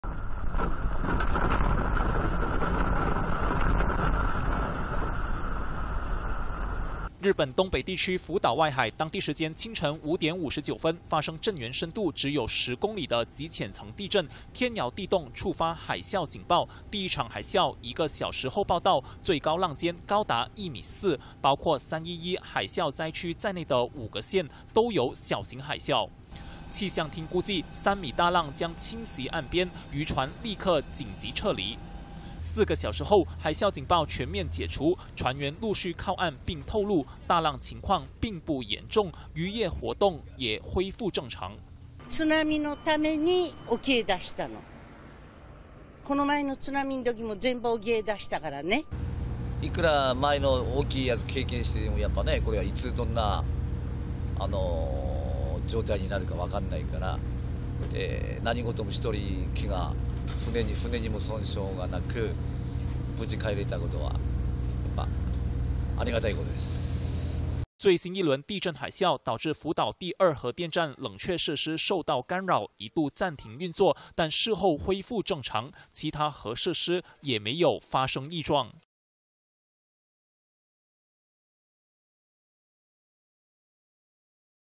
七级强震重临福岛 海啸警报惊心四小时 [20161122]
日本311大海啸重灾区，在我国时间今天清晨5点再次发生大地震。福岛外海7.4级强震引发小海啸，还一度干扰核电站运作。虽然海啸警报在地震四小时后解除，但有两人蒙受轻伤。